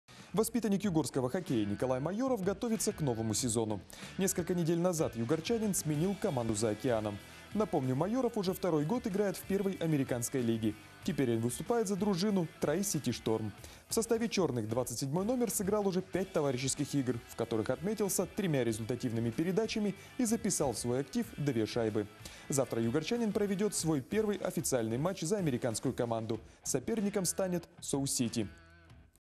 Югорский хоккеист готовится к новому сезону за океаном
Воспитанник югорского хоккея Николай Майоров готовится к новому сезону. Несколько недель назад югорчанин сменил команду за океаном.